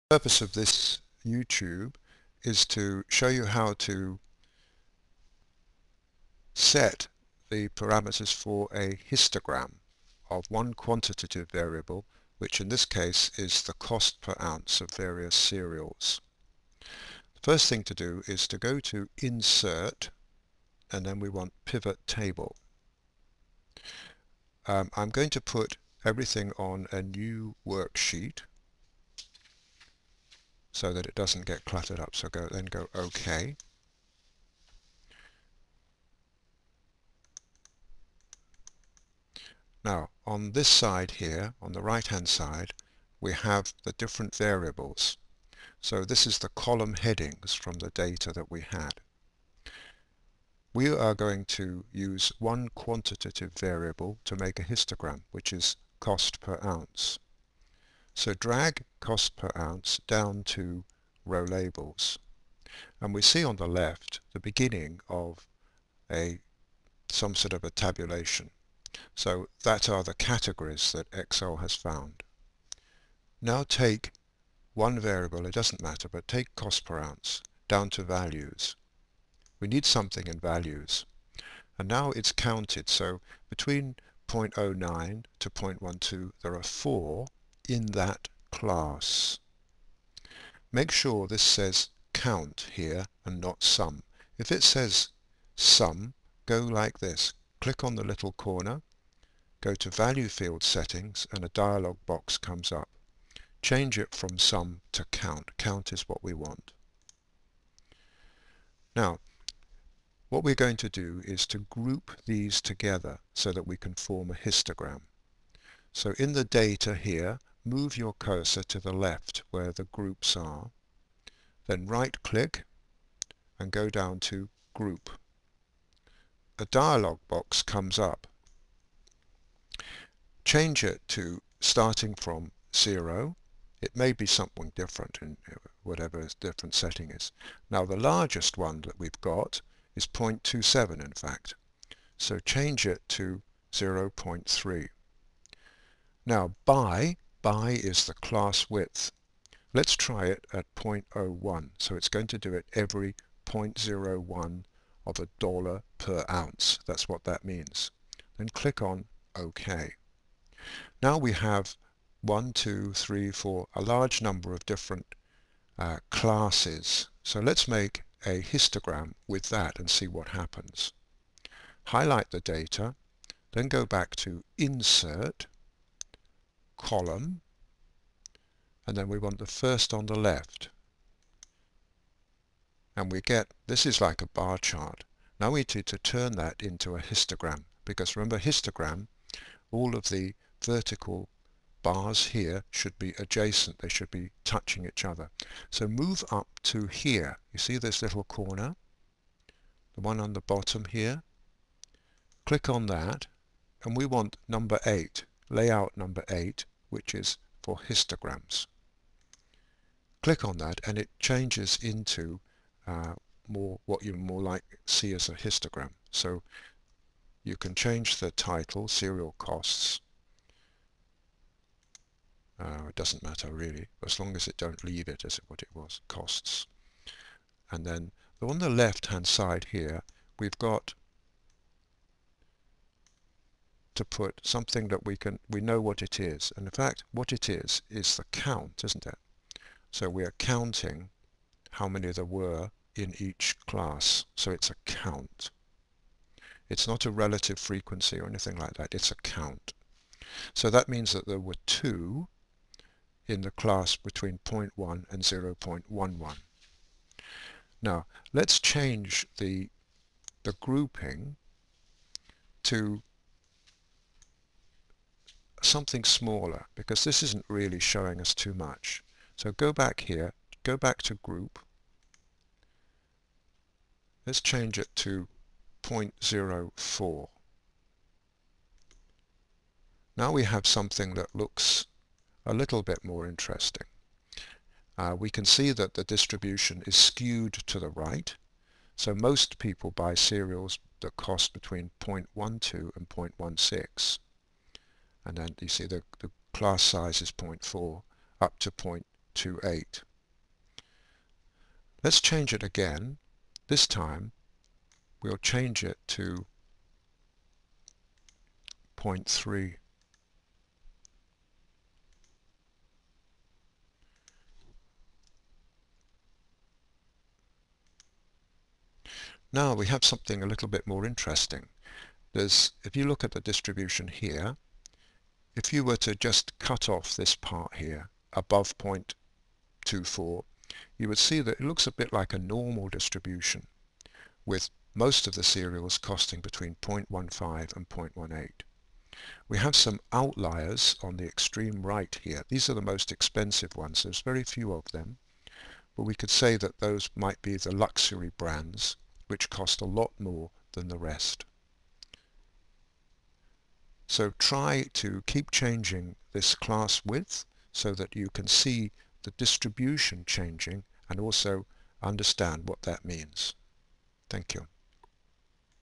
Histogram (Cereals) Changing Class-width.avi
Creating a histogram using Excel's pivot table, and changing the class width